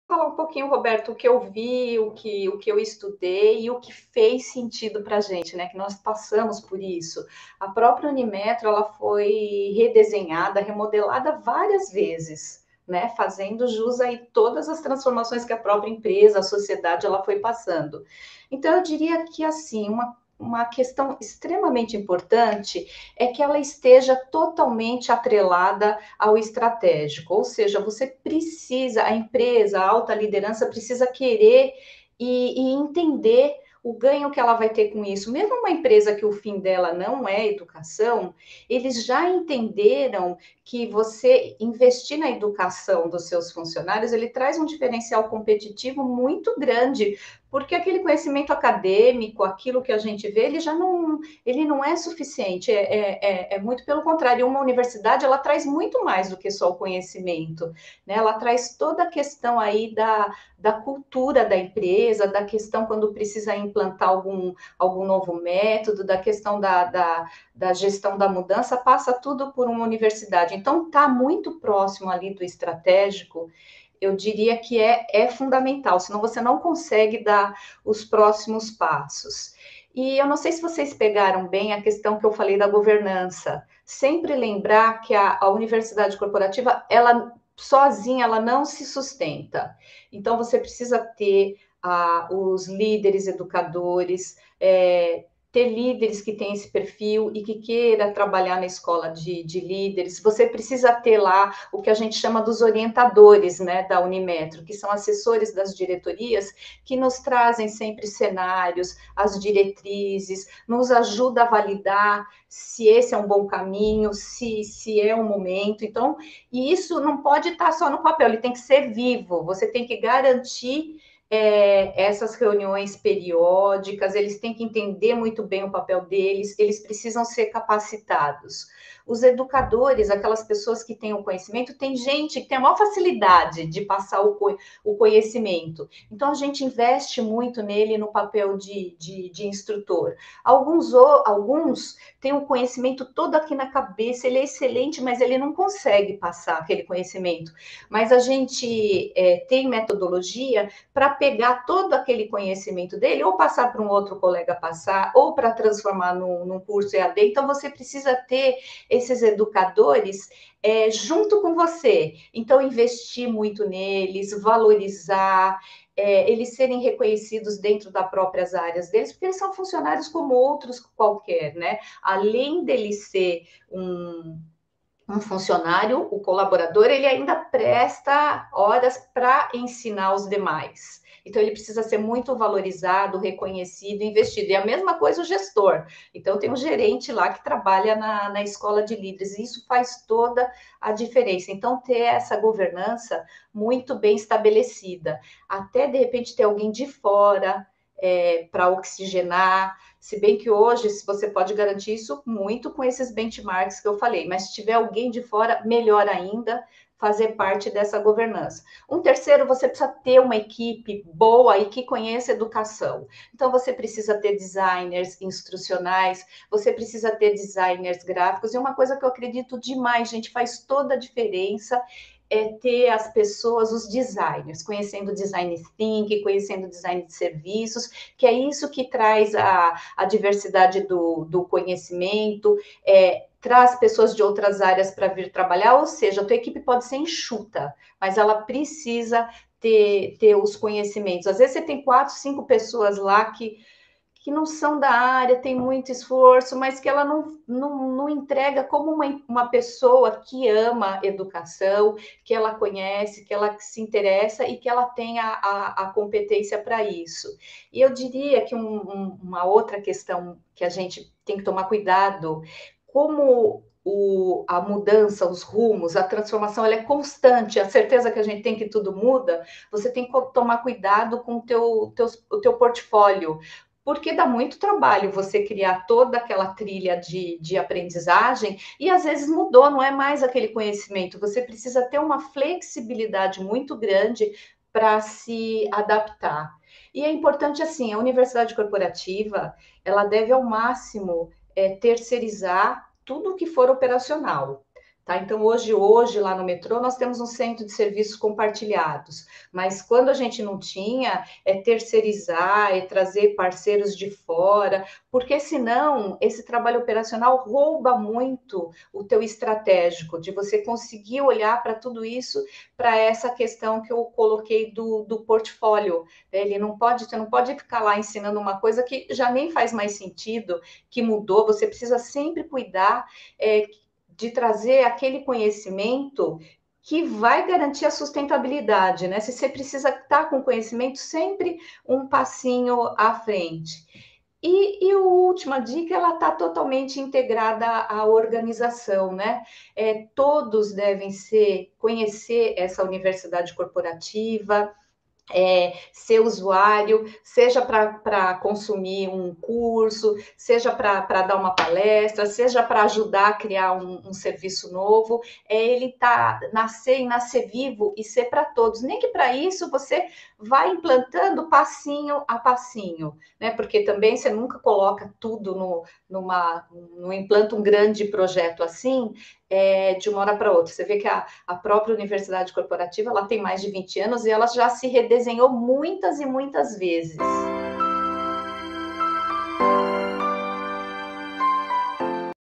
DICAS VALIOSAS PARA IMPLANTAR UMA UNIVERSIDADE CORPORATIVA
Silvia dá várias dicas importantes para uma implantação de sucesso, como estar atrelada ao estratégico da empresa; a governança bem estabelecida e os papéis dos gestores, educadores, facilitadores bem definidos; além de uma equipe boa que conheça educação; atualização constante do portfólio de cursos; e terceirizar o máximo do serviço operacional; todos integrados a Universidade Corporativa.